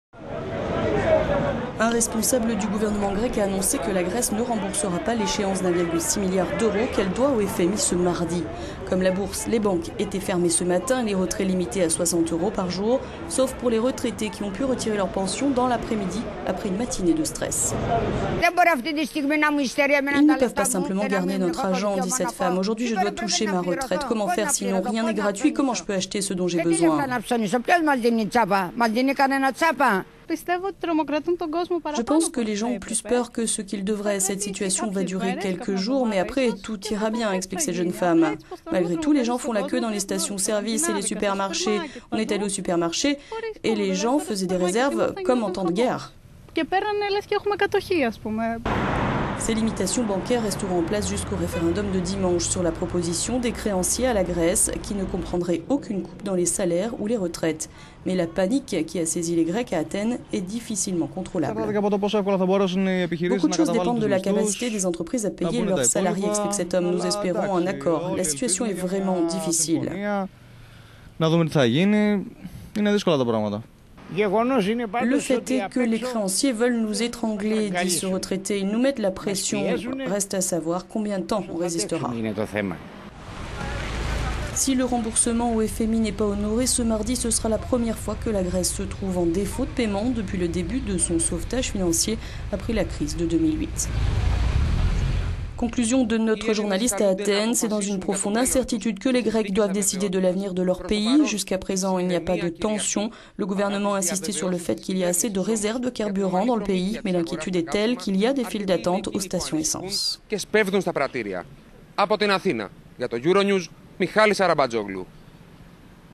Les Grecs dans une profonde incertitude
Comme la Bourse, les banques étaient fermées ce matin, et les retraits limités à 60 euros par jour. Sauf pour les retraités qui ont pu retirer leurs pensions dans l'après-midi après une matinée de stress :

"_Ils ne peuvent pas simplement garder notre argent. Aujourd'hui, je dois toucher ma retraite. Comment faire sinon ? Rien n'est gratuit, comment je peux acheter ce dont j'ai besoin ?_"

"_Je pense que les gens ont plus peur que ce qu'il devrait. Cette situation va durer quelques jours, mais …